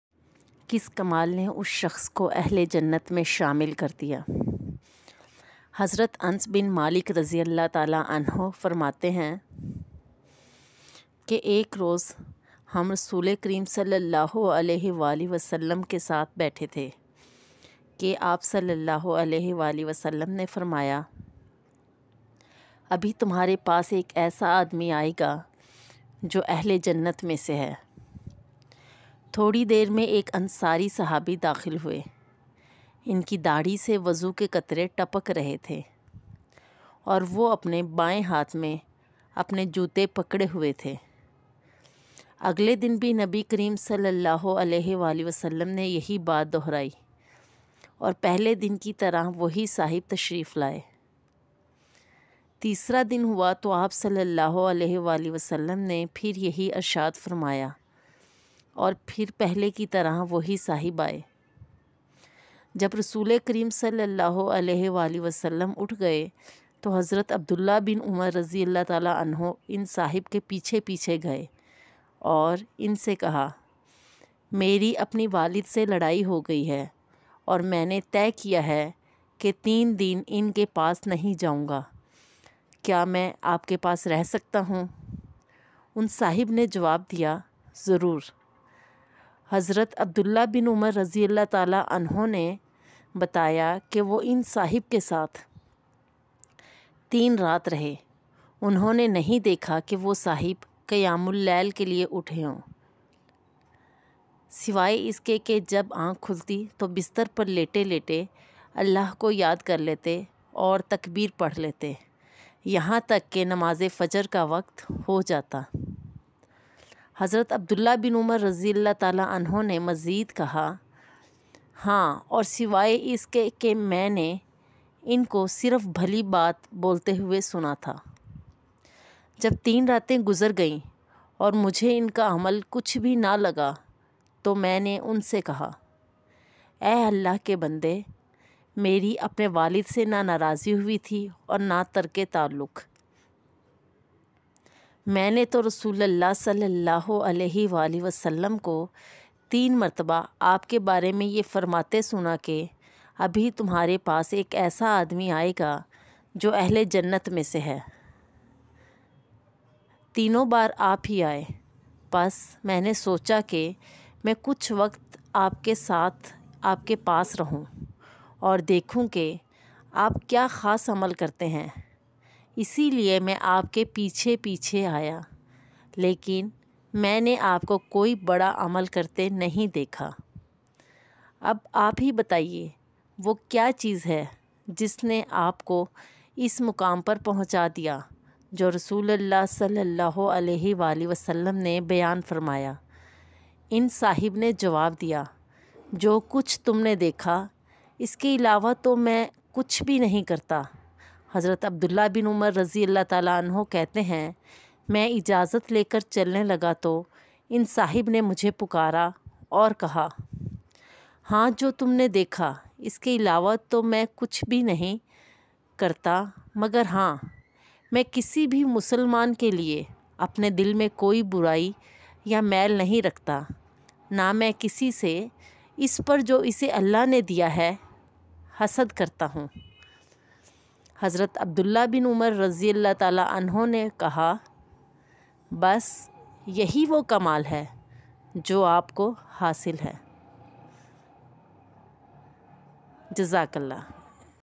Sahabi ka waqia/ true islamic story/ Hadees se manqool waqiat. کس کمال نے صحابی کو اہل جنت بنا دیا ؟
true islamic story
سچی اسلامی کہانیاں
islamic waqia
اسلامی واقعہ
Sahabi k amAl
صحابی کا عمل

Hazrat Muhammad SAW ki basharat
حضرت محمد صلی اللہ علیہ وسلم کی بشارت
Hazrat Abdullah bin Umar ka tajsus or maloom krna
حضرت عبداللہ بن عمر کا تجسّس اور معلومات کرنا
dosry momin k liye dil ko saf rakhna
دوسرے مومن کے لیے دل کو صاف رکھنا
Hasad se bachna or uska ajar
حسد سے بچنا اور اس کا اجر
Hadees se makhooz waqiat
حدیث سے ماخوذ روایت